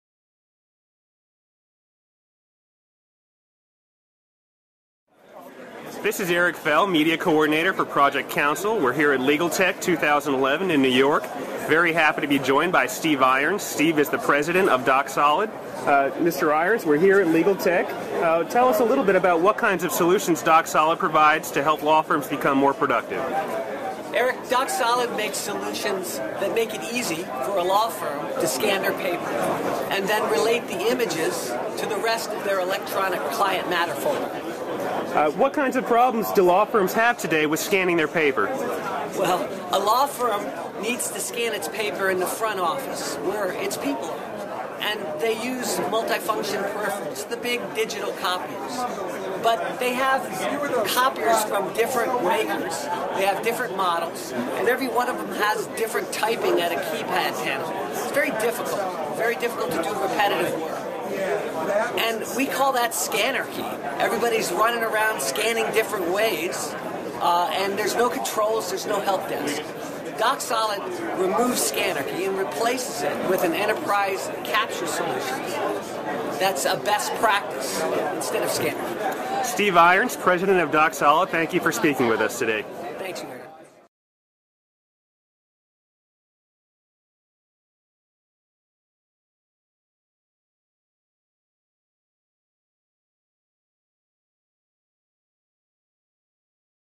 LegalTech New York 2011: DocSolid
A chat with Steve Irons, President of DocSolid, at LegalTech 2011